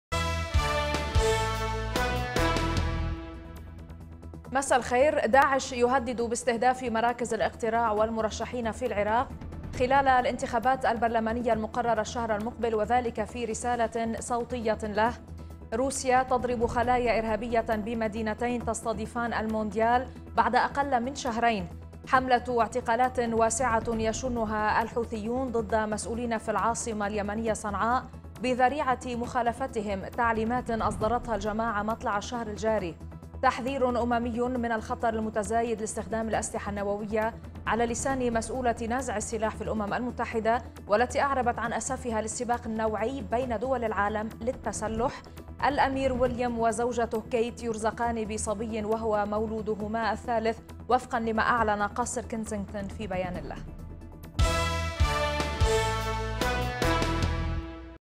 الأمير وليام وزوجته كيت يرزقان بصبي وهو مولودهما الثالت.. ضمن "الأخبار في دقيقة"
شاهد آيضا:
قيادي بعثي يفضح تاريخ آل الأسد!؟

- داعش يهدد باستهداف مراكز الاقتراع والمرشحين في العراق خلال الإنتخابات البرلمانية المقررة الشهر المقبل، وذلك في رسالة صوتية له.

- روسيا تضرب خلايا إرهابية بمدينتين تستضيفان المونديال مبارياتِ بطولة كأس العالم بعد أقل من شهرين.

- حملة اعتقالات واسعة يشنها الحوثيون ضد مسؤولين في العاصمة اليمنية صنعاء بذريعة مخالفتهم تعليمات أصدرتها الجماعة مطلع الشهر الجاري.

- تحذير أممي من الخطر المتزايد لاستخدام الأسلحة النووية على لسان مسؤولة نزع السلاح في الأمم المتحدة والتي أعربت عن أسفها للسباق النوعي بين دول العالم للتسلح.

- الأمير وليام وزوجته كيت يرزقان بصبي وهو مولودهما الثالت وفقا لما أعلنه قصر كنسينغتون في بيان له.